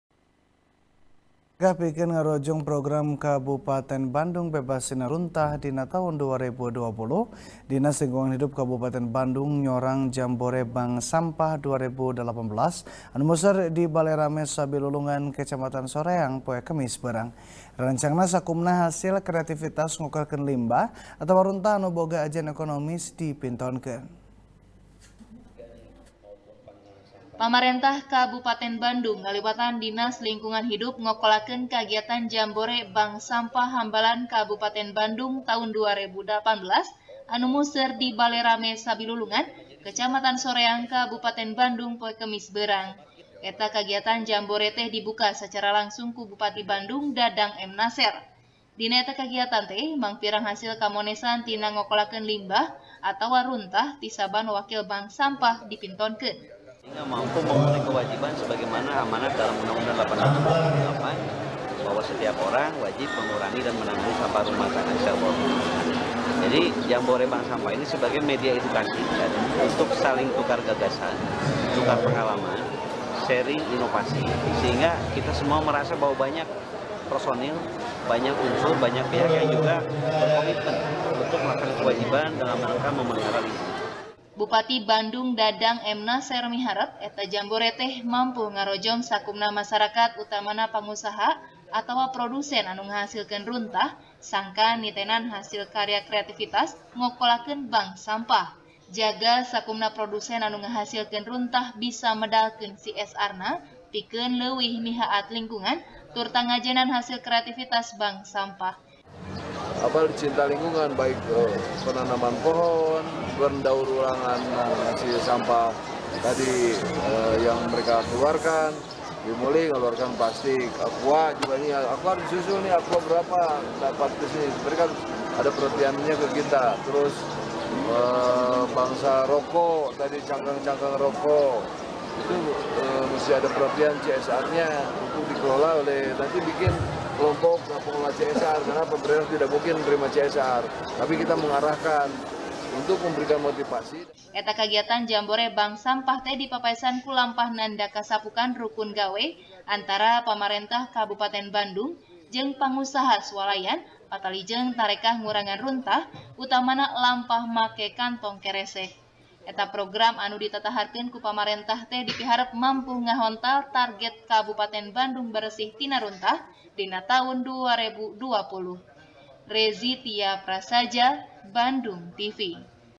Bebas Sampah 2020, Pemkab Bandung Nyorang Jambore Bank Sampah | TP | BANDUNGTV
TP | BANDUNGTV | DERI FIRMAN | Minangka tarekah pikeun ngarojong program kabupaten bandung bebas tina runtah dina taun dua rebu dua puluh, dinas lingkungan hidup kabupaten bandung nyorang jambore bank sampah dua rebu dalapan belas, anu museur di bale rame sabilulungan, kecamatan soreang, poe kemis beurang. Rarancangna, sakumna hasil kreativitas ngokolakeun limbah atawa runtah anu boga ajen ekonomis dipintonkeun.